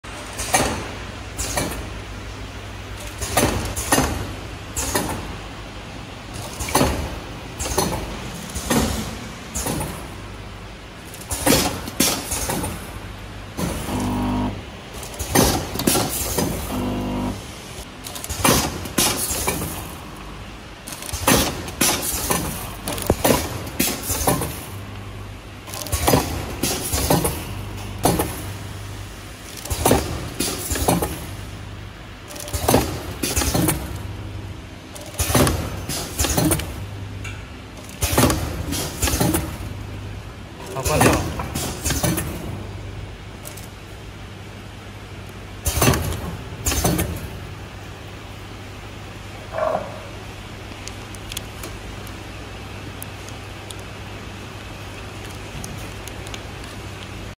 Liquid + Grain Pouch Packing Machine
HI, There
Tks for watch our channel again.
In this video, we are showing how to packing grain and liquid in one machine, so that we can meet packing 2 different material in one same pouch.

PackHome Co.,Ltd
Huno.Tsai
Skype:huno.tasi